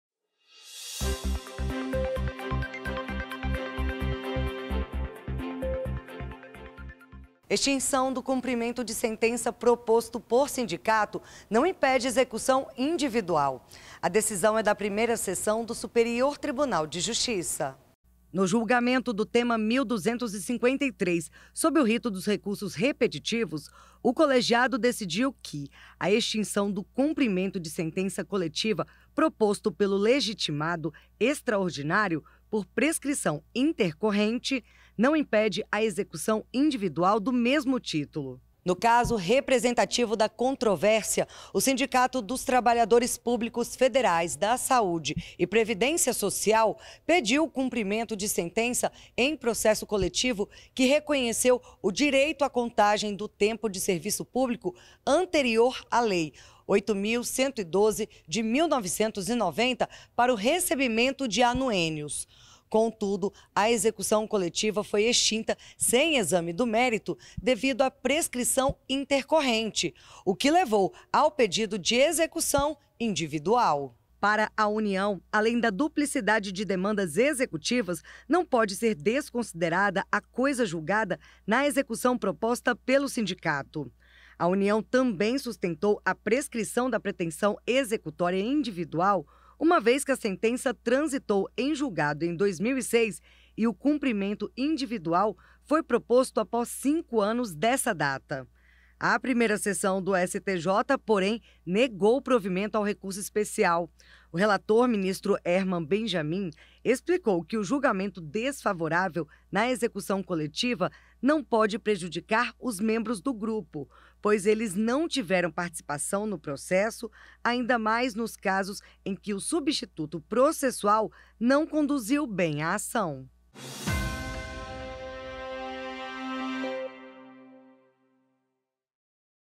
Extinção do cumprimento de sentença proposto por sindicato não impede execução individual
​No julgamento do Tema 1.253, sob o rito dos recursos repetitivos, a Primeira Seção do Superior Tribunal de Justiça (STJ) decidiu que "a extinção do cumprimento de sentença coletiva proposto pelo legitimado extraordinário, por prescrição intercorrente, não impede a execução individual do mesmo título".
REsp 2078485
REsp 2078989
REsp 2079113